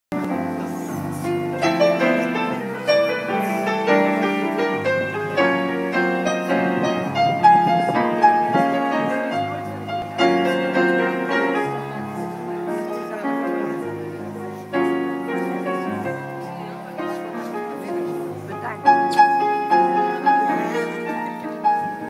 Johan Hoogewijs
Juni 2013 herdenking Kaat Tilley, Marc Vermeulen met Liesbeth Wouters